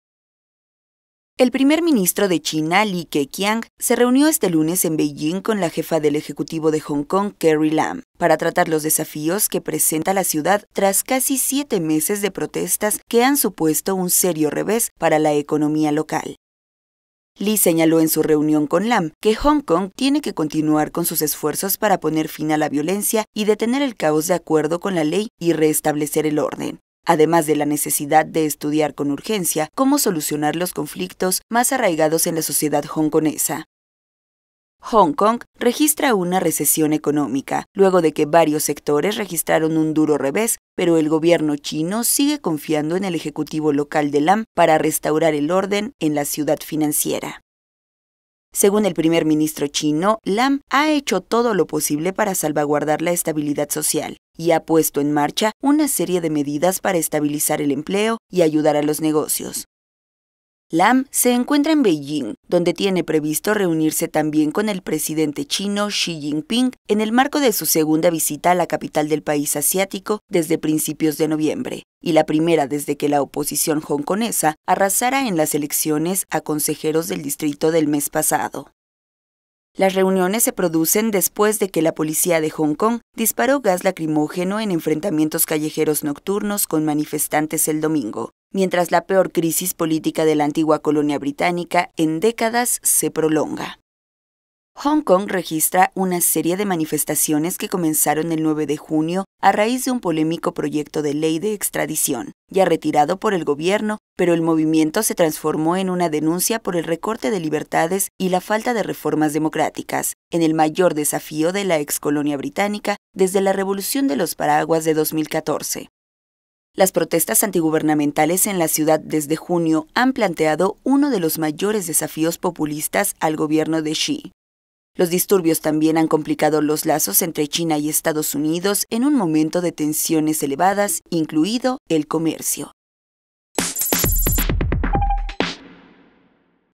China pide a jefa del Ejecutivo de Hong Kong acabar con protestas
El primer ministro de China, Li Keqiang, se reunió este lunes en Beijing con la jefa del Ejecutivo de Hong Kong, Carrie Lam, para tratar los desafíos que presenta la ciudad tras casi siete meses de protestas que han supuesto un "serio revés" para la economía local.